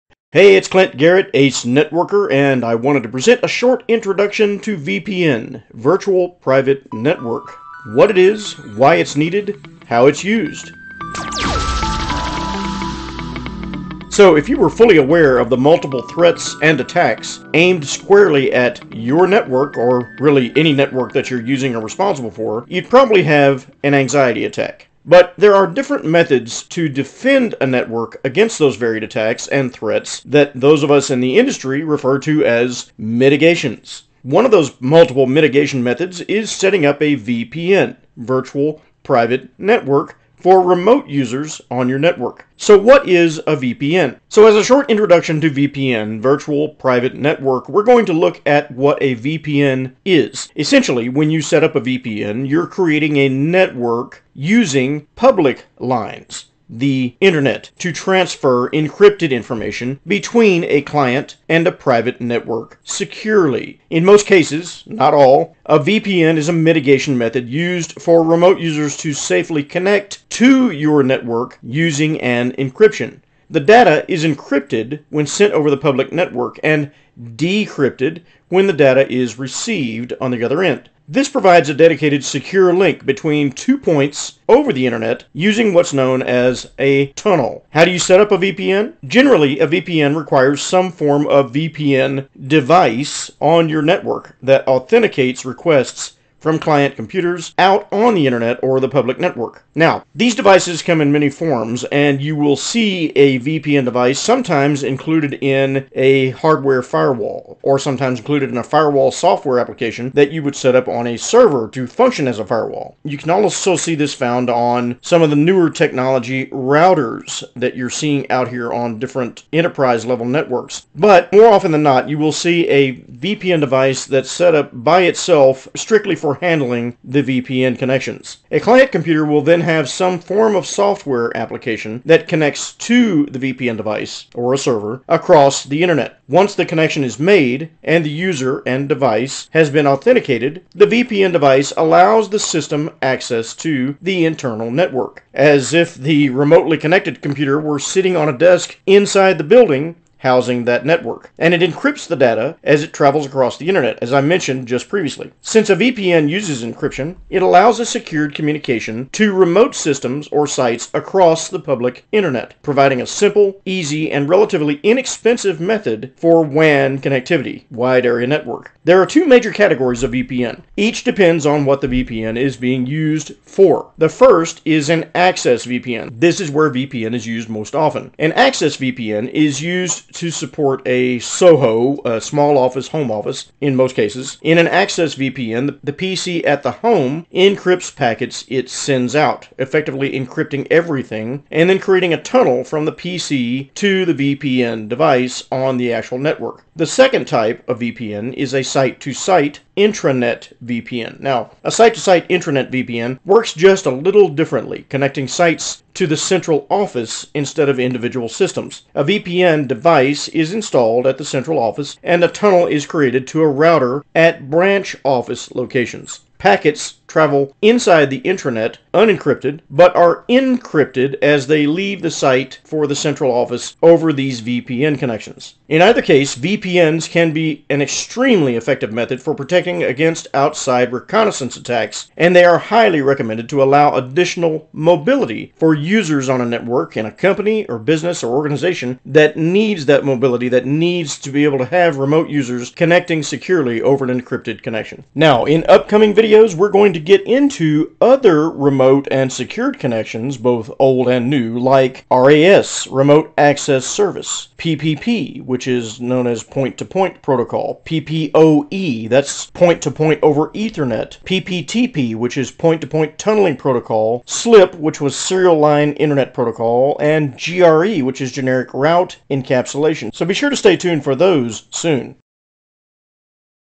Intro to VPN (Virtual Private Network)
Introduction to VPN (Virtual Private Network)...What is a VPN? Why do we need a VPN? Going to answer these questions and more in this short video